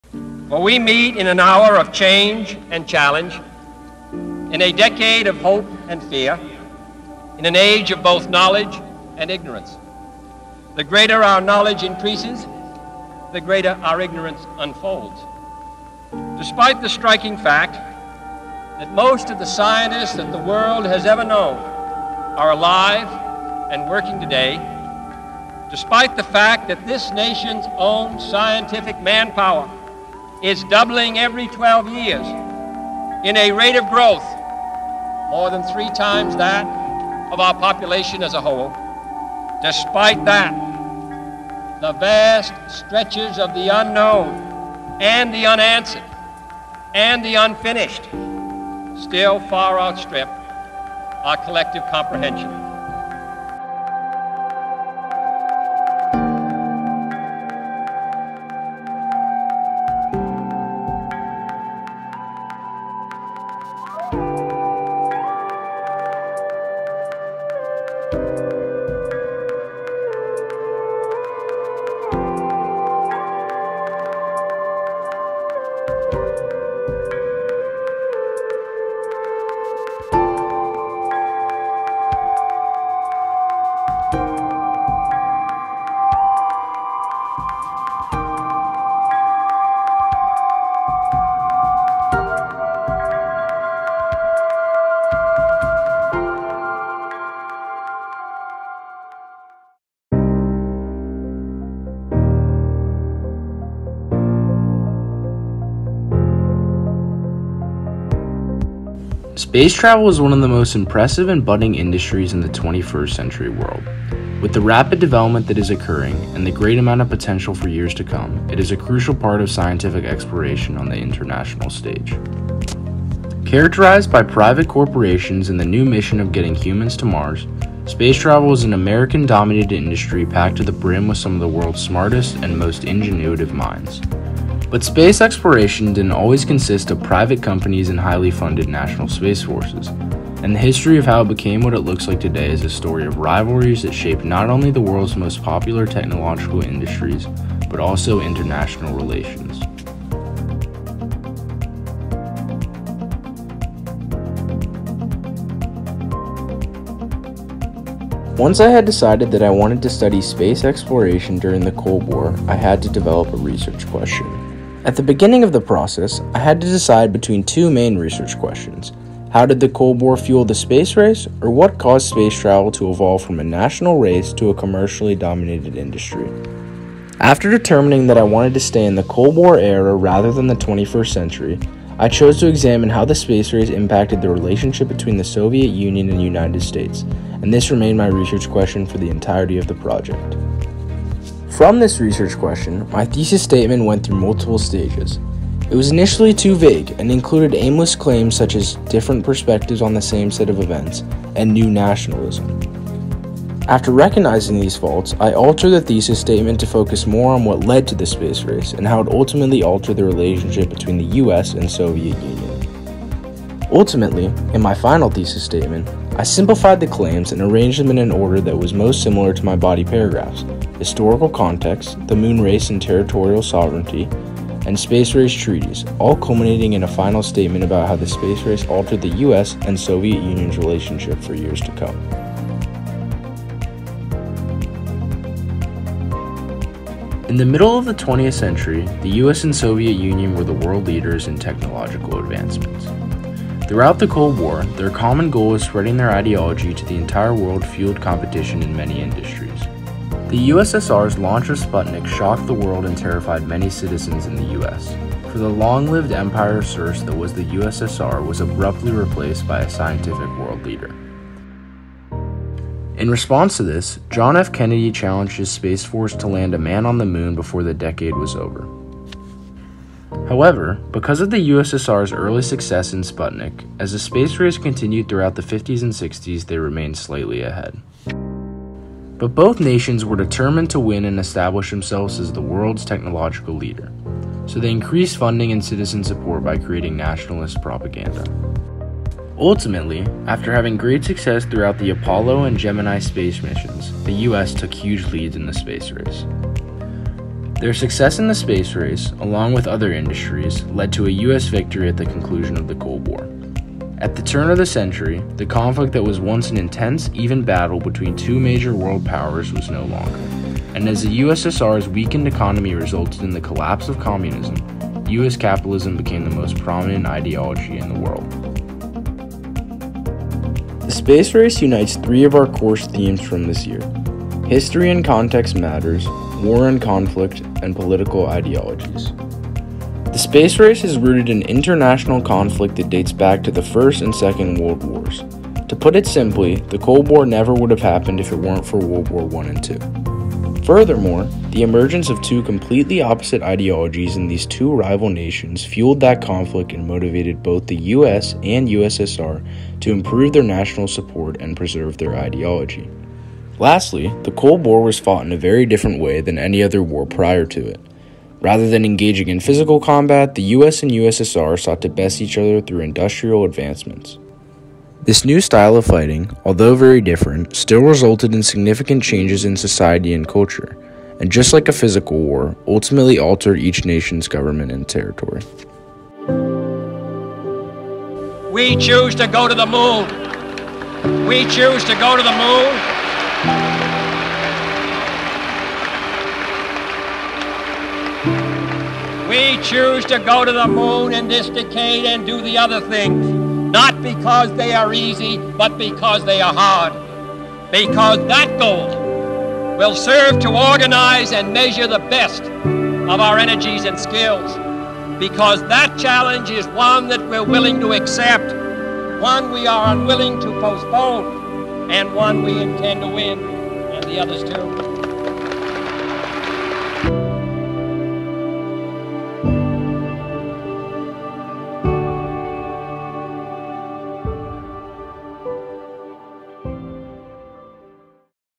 The Space Race | MWH Final Project | Andrew Lay
This is my cumulative academic experience about my final essay on how the space race altered the political and economic relationship between the US and Soviet Union.